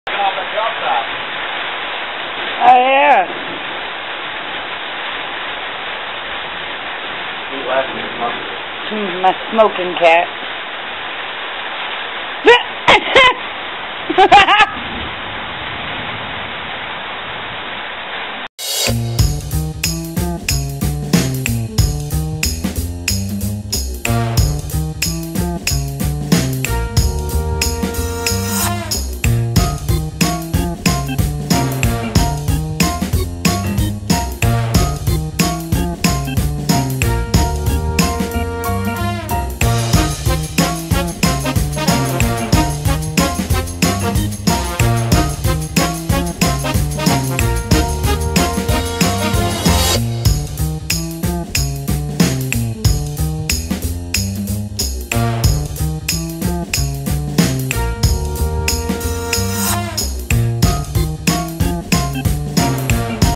Kitten Smokes A Cigarette
An adorable kitten plays with a cigarette and smokes it.